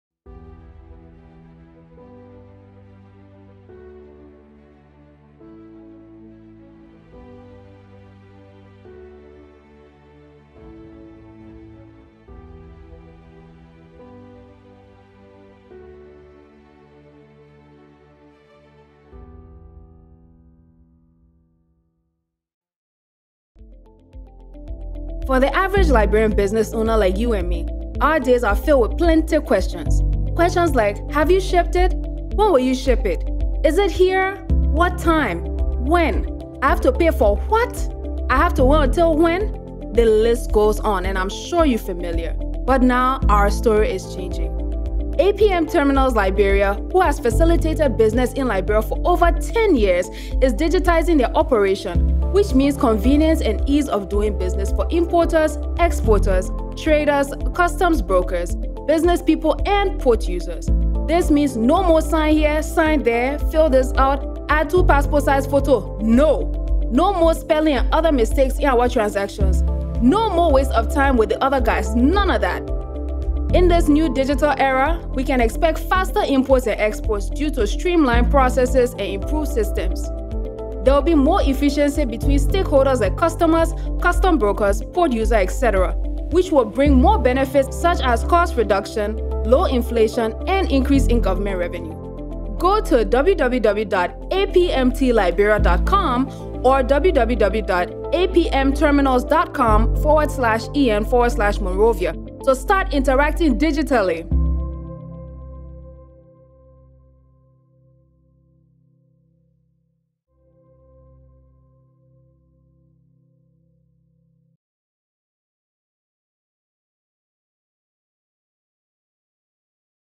The Freeport of Monrovia has gone PAPERLESS!
APM Terminals Liberia Ltd. in collaboration with Liberia Revenue Authority (LRA), the Ministry of Finance, Planning and Development, Ministry of Commerce and Industry, National Port Authority (NPA), Central Bank of Liberia, Custom Brokers Association, MTS, LBDI, Ecobank, GT Bank, Maersk, CMA CGM, MSC and customers of the Free Port launched the Port Automation and Digitization (PAD) project.

This will significantly improve the efficiency of both custom clearance processes and terminal operations in the long term and reaffirm the Free Port of Monrovia’s position as the gateway to the economy of Liberia and the Mano River Union.

📽FILIFILI STUDIOS